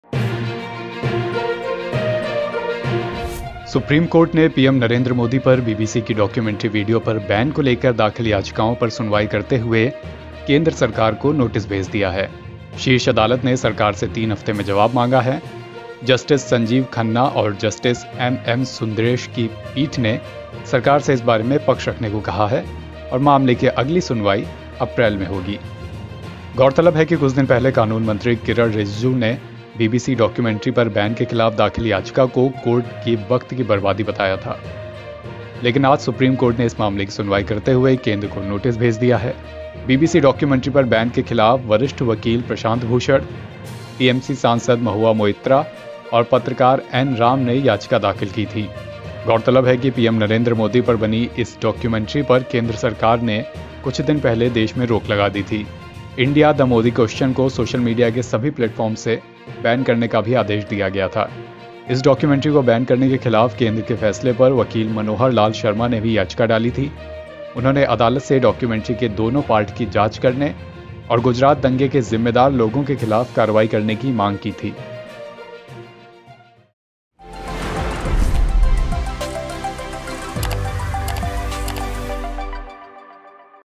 BBC Modi Documentary Ban के खिलाफ लगी याचिकाओं पर सुप्रीम कोर्ट ने केंद्र से क्या कहा
Supreme Court hears petitions against BBC Modi Documentary Ban and asks center to file response.